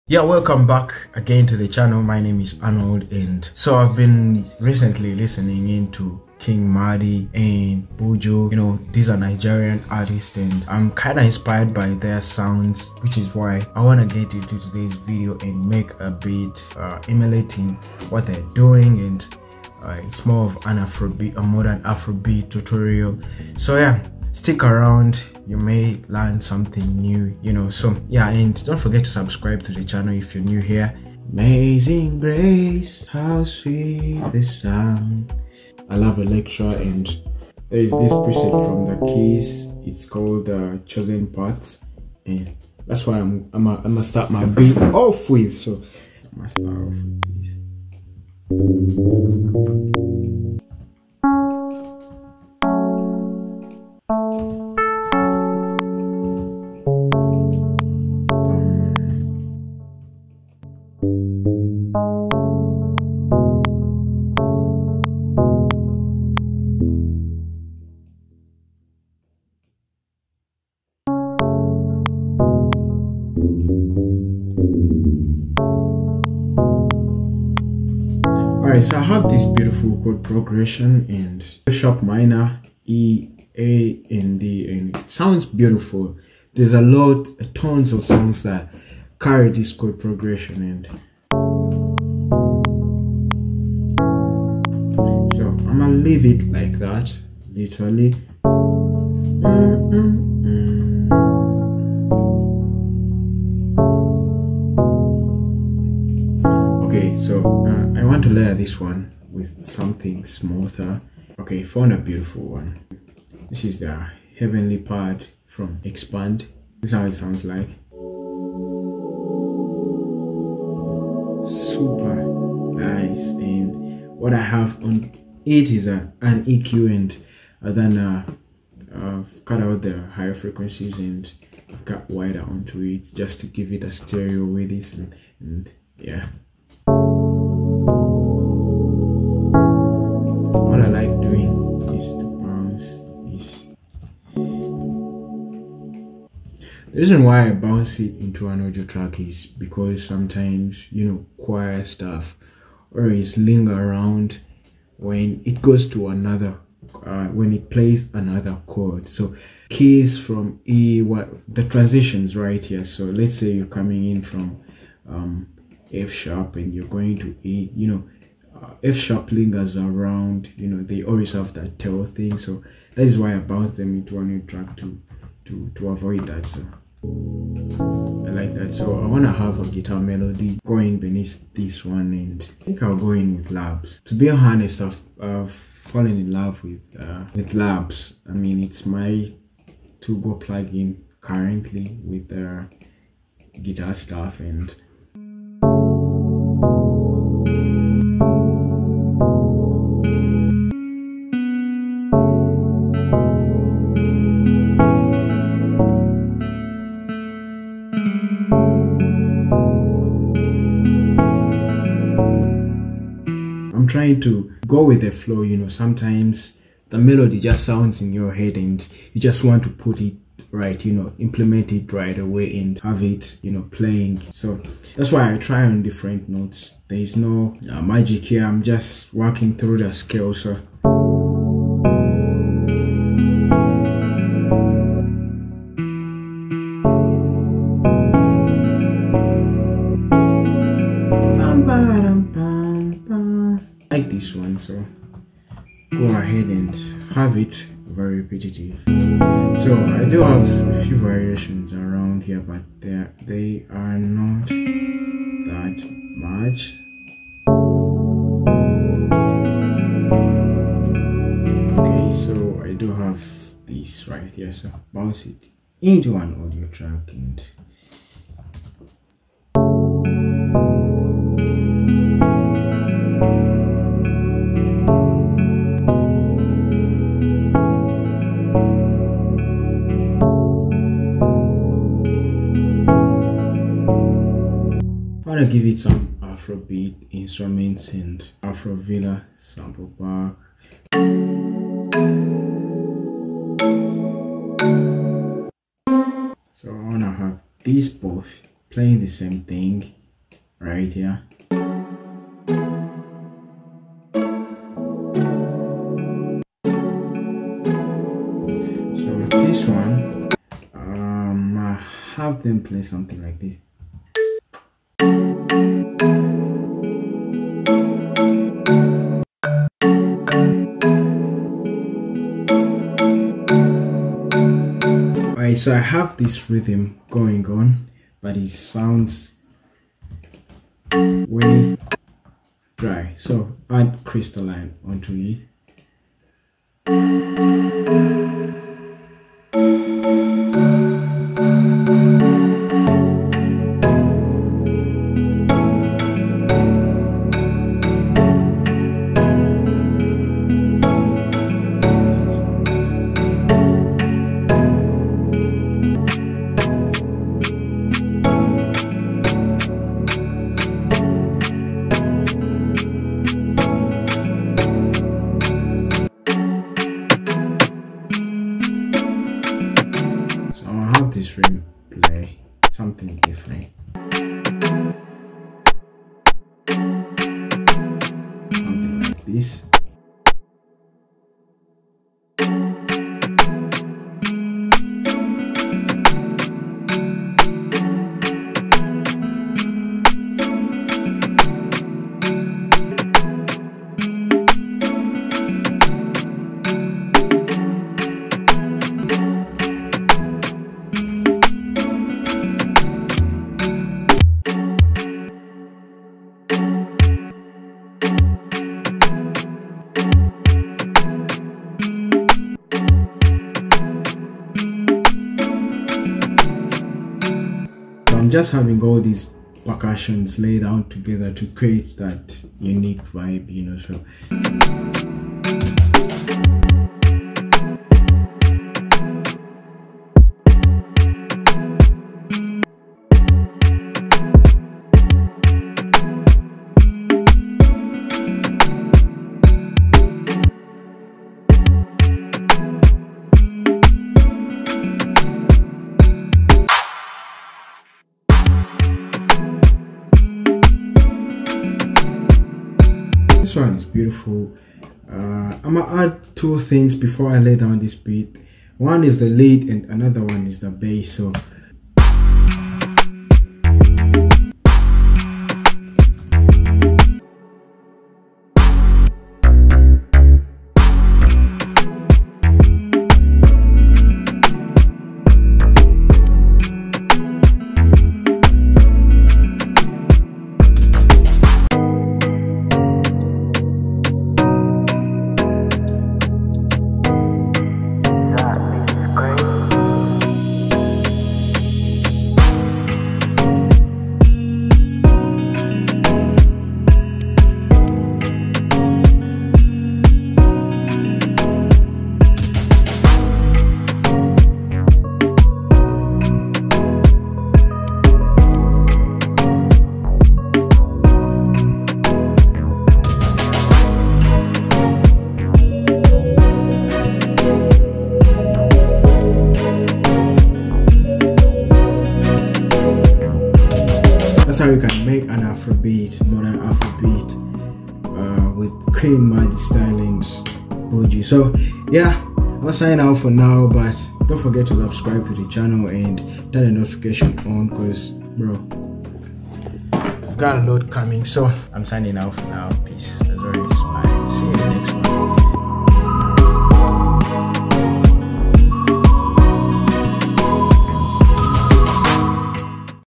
How to Make Modern Afro Beat 2024 | Studio One Tutorial
In this Video I show you how I make Afro beats in Studio one
inspired by sounds of Qing Madi and BNXN and so many Afro beat Artists that fall in this Category.
I have free Melodies Kits and you can Download